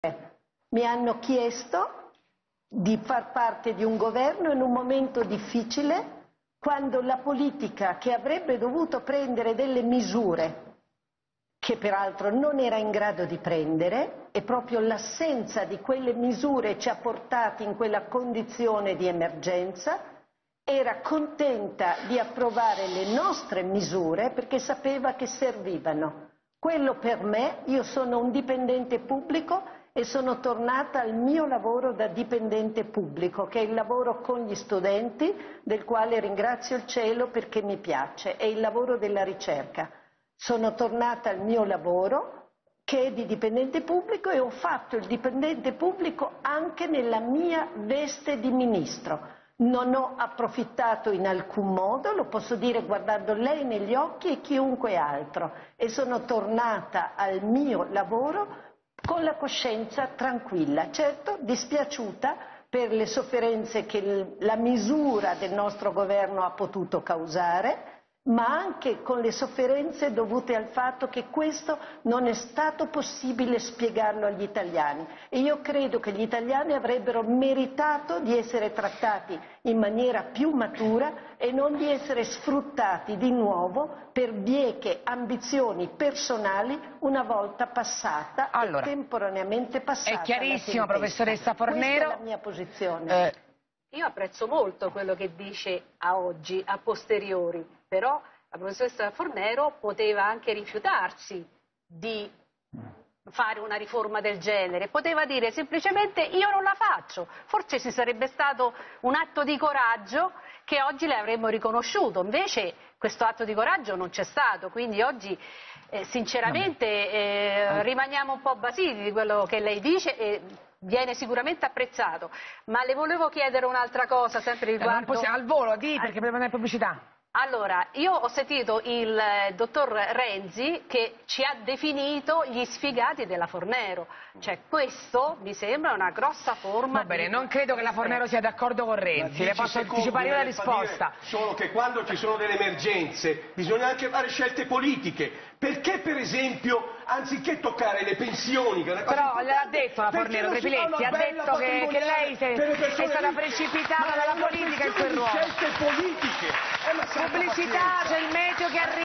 Elsa Fornero risponde alla domanda: 'Ma chi te lo ha fatto fare?'
In studio Paola Picca, una lavoratrice precoce che per la legge Fornero ha perso lavoro e pensione, pone alla professoressa la domanda fatidica che forse in molti si sono fatti già ai tempi delle sue lacrime in tv, la quale replica: "Sono funzionario pubblico, ho fatto solo il mio dovere"